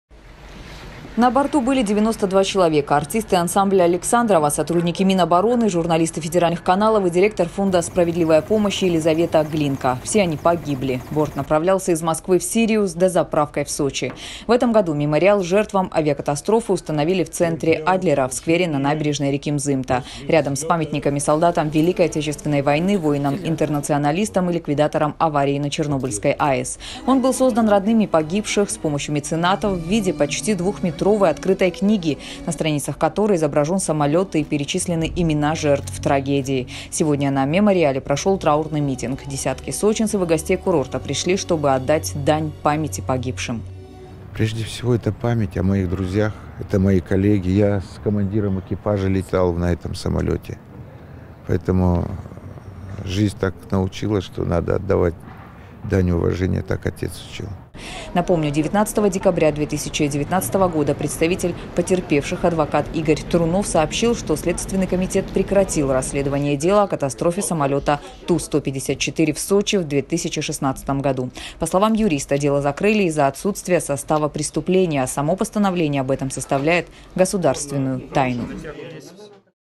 Сегодня в Сочи вспоминают жертв трагедии, которая произошла у берегов курорта три года назад
25 декабря 2016 года самолет  Ту-154 Минобороны разбился под Сочи.

На борту были 92 человека - артисты ансамбля Александрова, сотрудники Минобороны, журналисты федеральных каналов и директор фонда «Справедливая помощь» Елизавета Глинка. Все они погибли. Борт направлялся из Москвы в Сирию с дозаправкой в Сочи.

В этом году мемориал жертвам авиакатастрофы установили в центре Адлера, в сквере на набережной реки Мзымта. Рядом с памятниками солдатам Великой Отечественной войны, воинам-интернационалистам и ликвидаторам аварии на Чернобыльской АЭС. Он был создан родными погибших с помощью меценатов, в виде почти двухметровой открытой книги, на страницах которой изображен самолет и перечислены имена жертв трагедии.

Сегодня на мемориале прошел траурный митинг. Десятки сочинцев и гостей курорта пришли, чтобы отдать дань памяти погибшим.